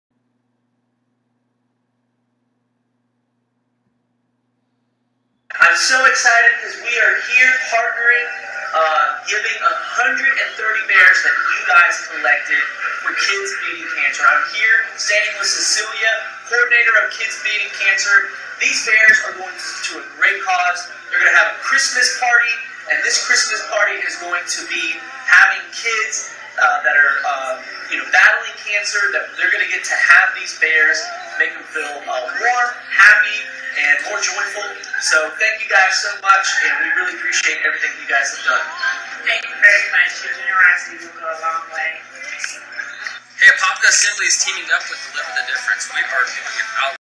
Kids Beating Cancer Teddy Bear Drive
Pastor from Apopka Assembly of God filmed the drop off teddy bears they collected for two great causes; helping out a UCF Cornerstone Team & Kids with life threatening illnesses.